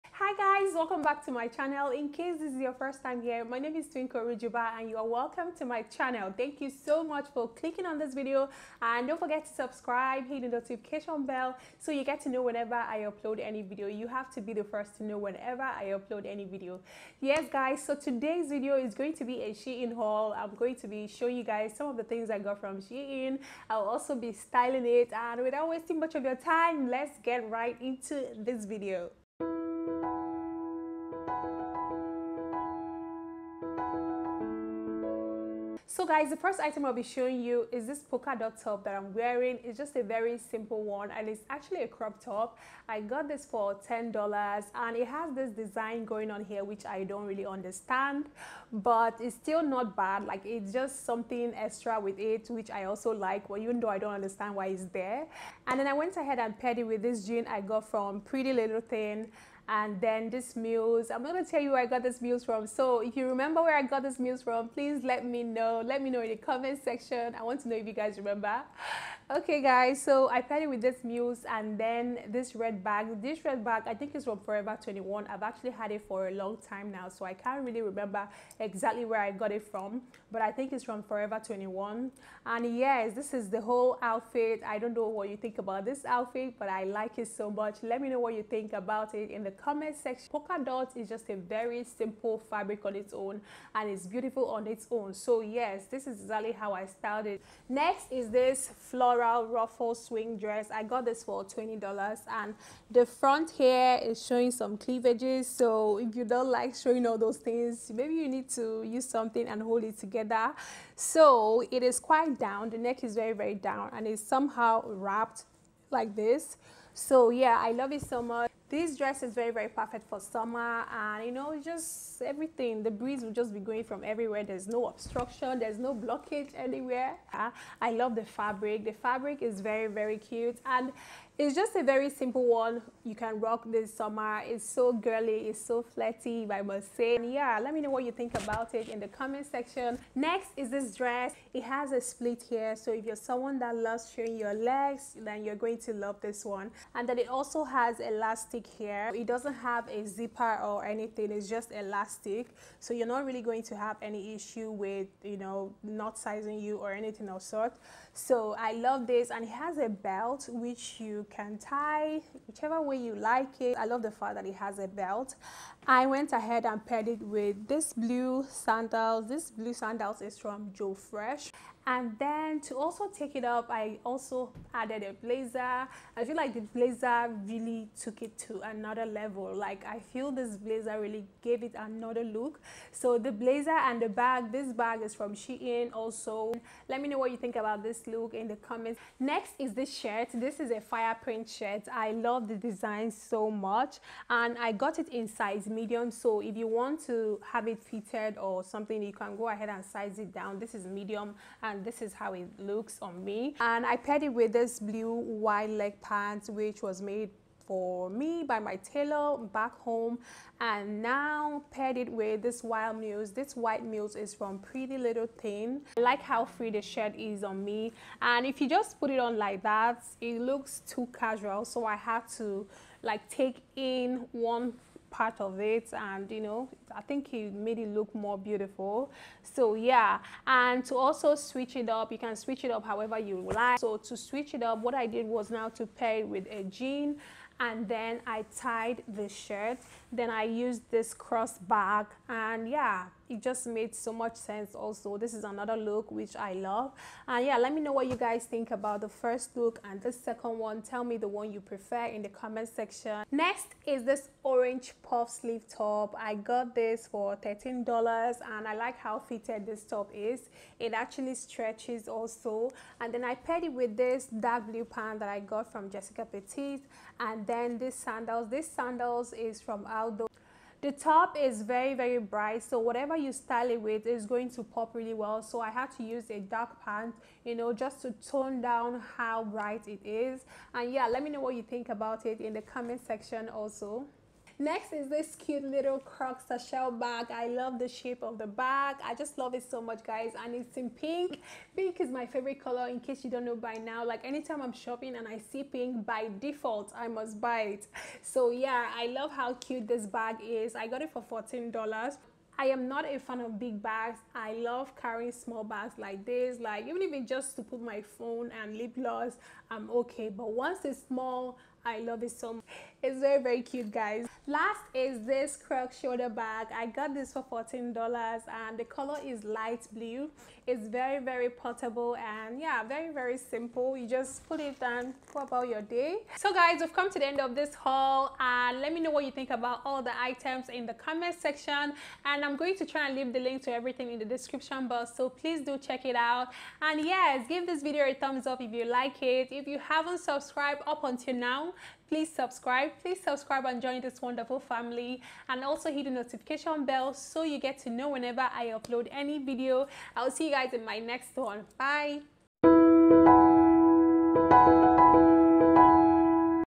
SHEIN SUMMER 2020 TRY-ON HAUL | SHEIN SUPER AFFORDABLE TRY ON HAUL
Hey Family, I'm back with another video, this one is SHEIN haul. Love you all.